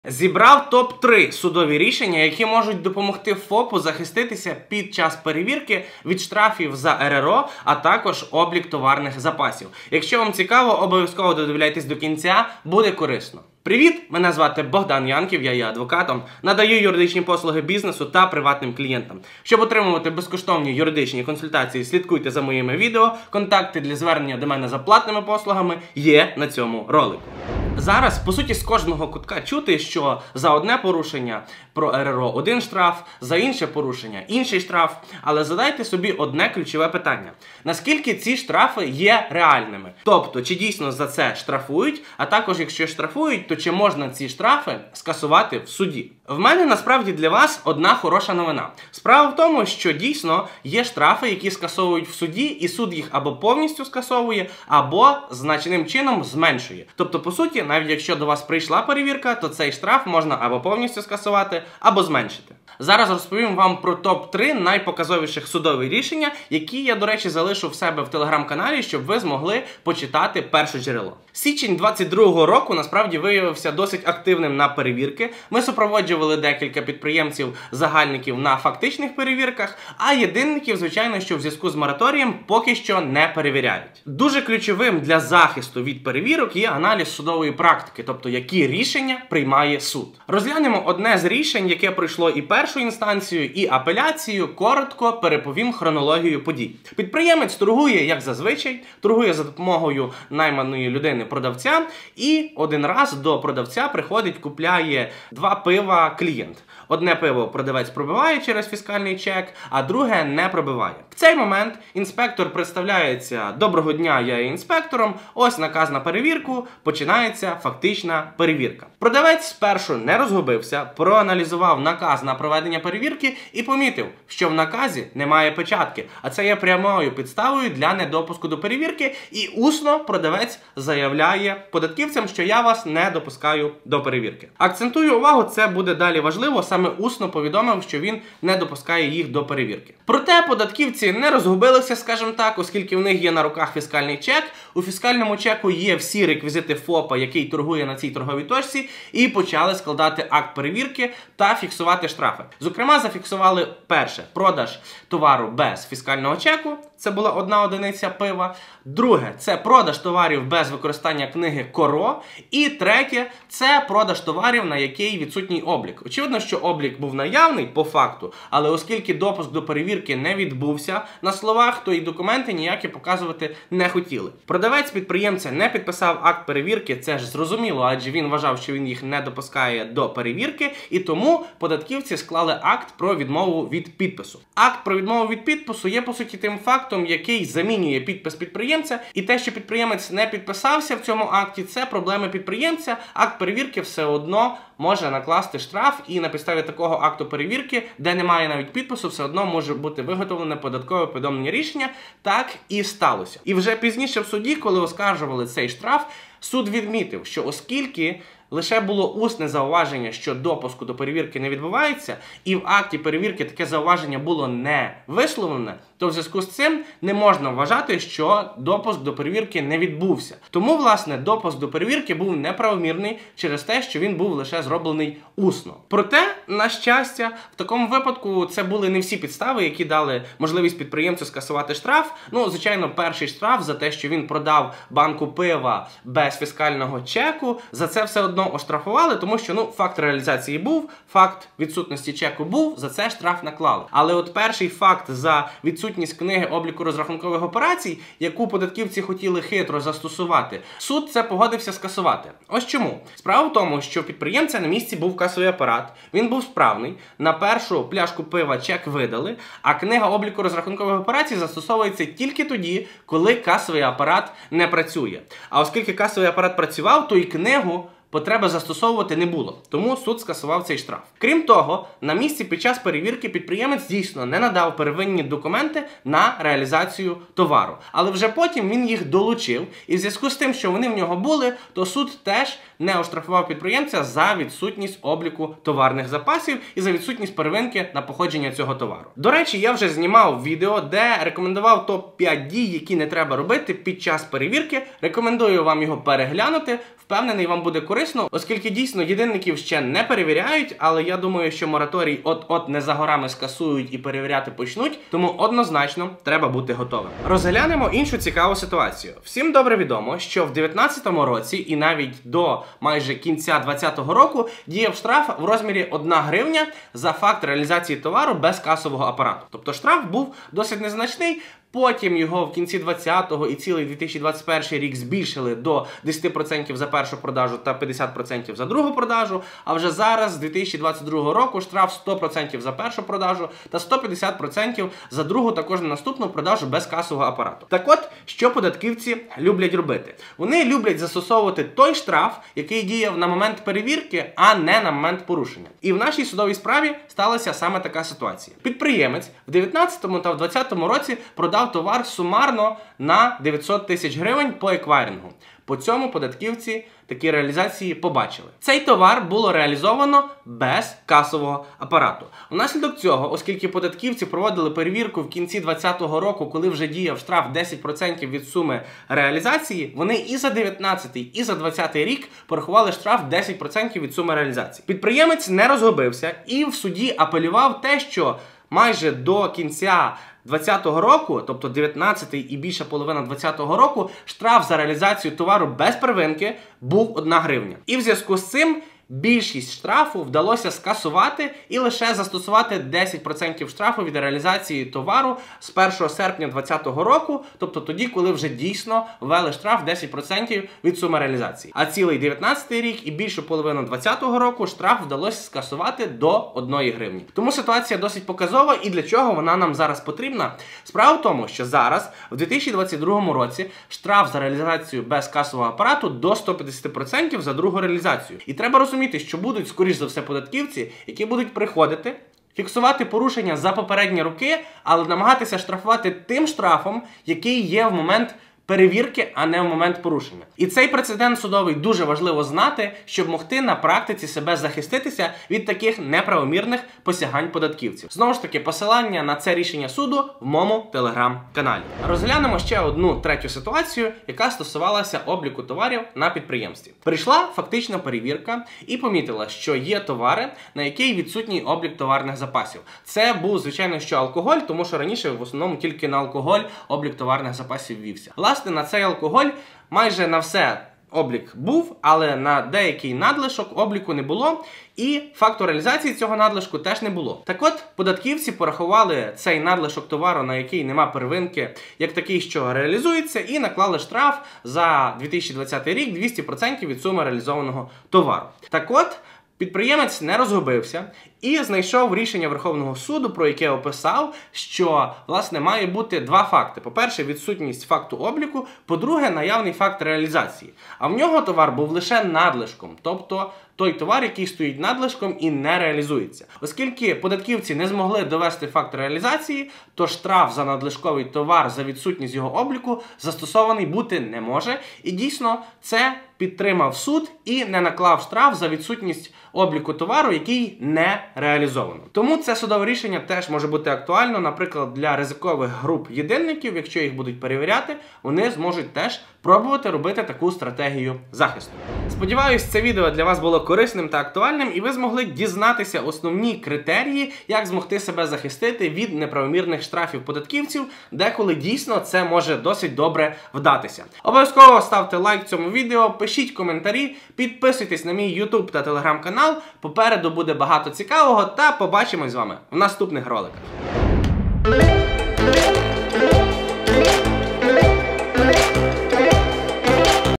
ТОП 3 судових рішення ФОП про РРО та облік товарів
ТОП 3 судових рішення ФОП про РРО та облік товарів ПЕРЕГЛЯНЬ🚀

00:00 - 00:24 Початок відео про ТОП 3 судових рішення ФОП про РРО та облік товарів
00:25 -  05:06 Які штрафи можна скасувати в суді. Ситуація №1
05:07 - 07:21 Які штрафи можна скасувати в суді. Ситуація №2
07:22 - 08:41 Які штрафи можна скасувати в суді. Ситуація №3
08:42 Завершення відео про ТОП 3 судових рішення ФОП про РРО та облік товарів